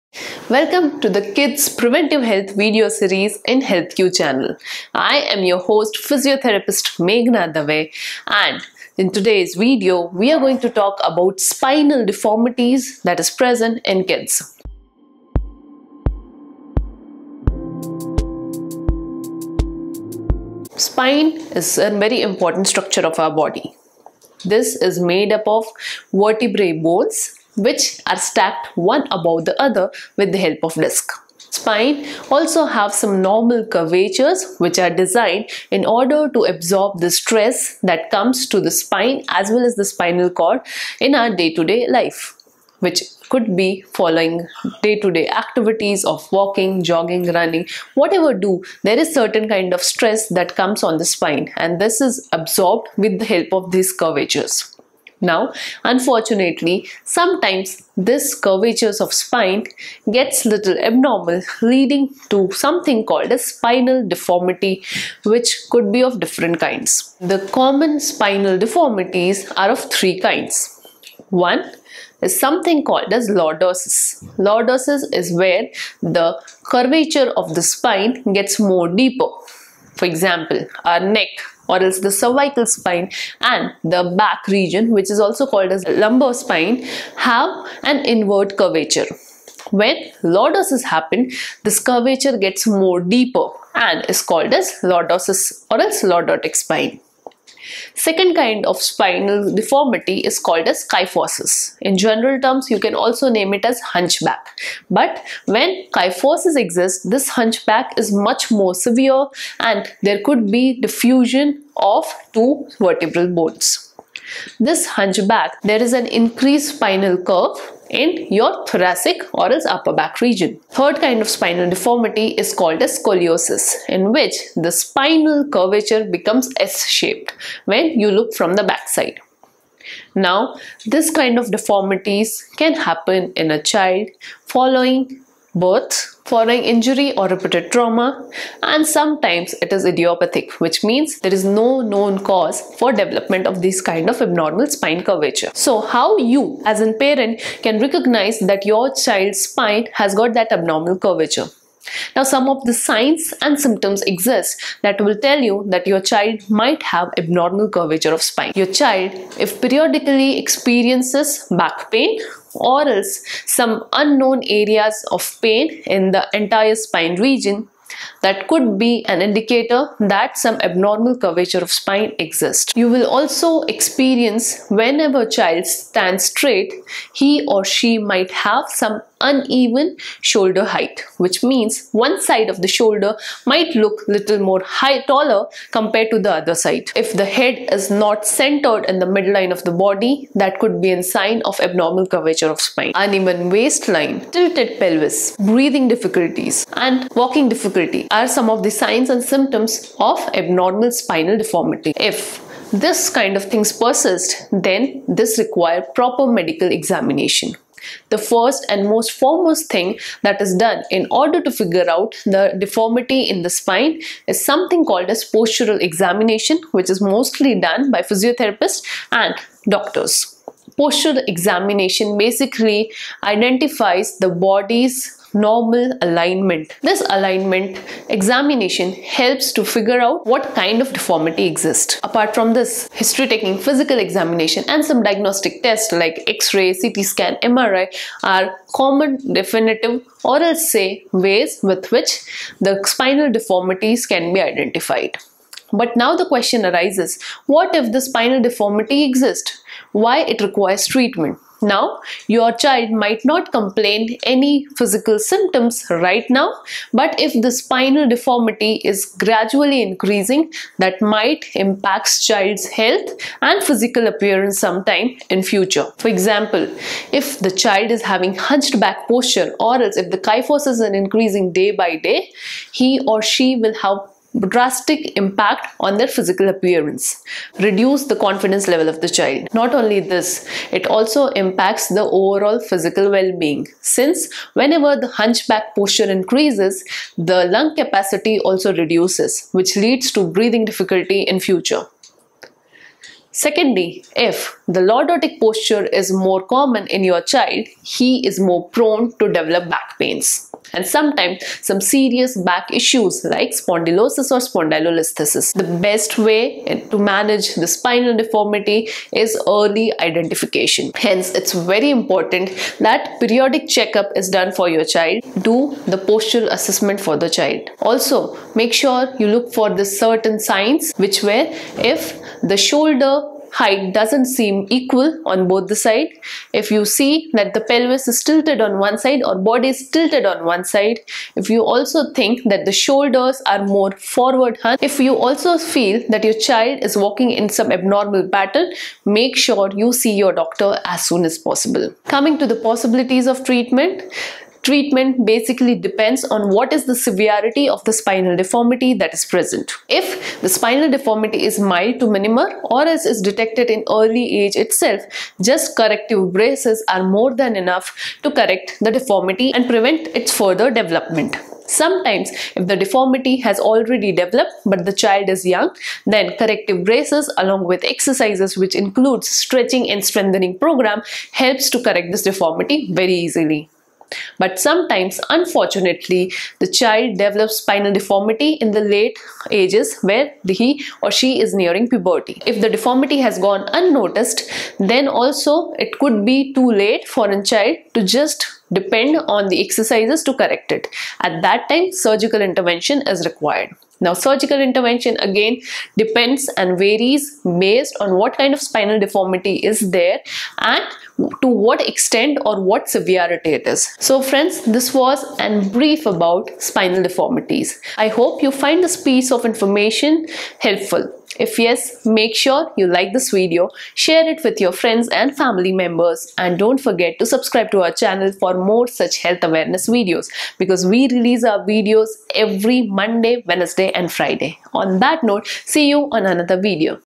3 COMMON SPINAL DEFORMITIES IN KIDS
3 COMMON SPINAL DEFORMITIES IN KIDS, this video explains about some of the common deformities that happens in kids , its implications and also give you an idea how it is managed.